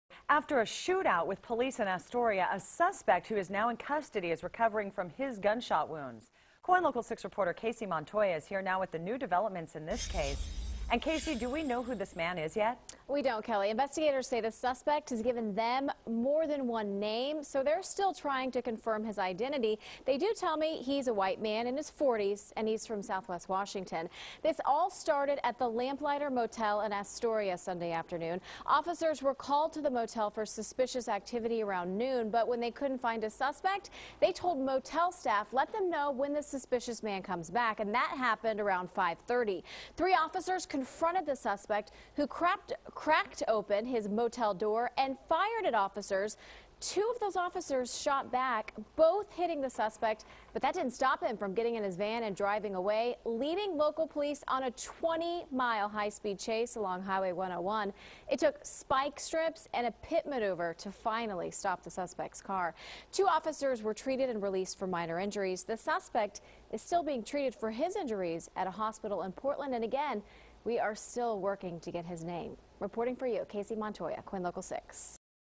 Suspect recovering following shootout with Astoria police
A man is recovering from a gunshot wound following a shootout with Astoria police.